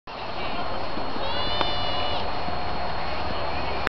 Mac on the Magic Carpet
Magic Carpet at Attitash. First day on skiis.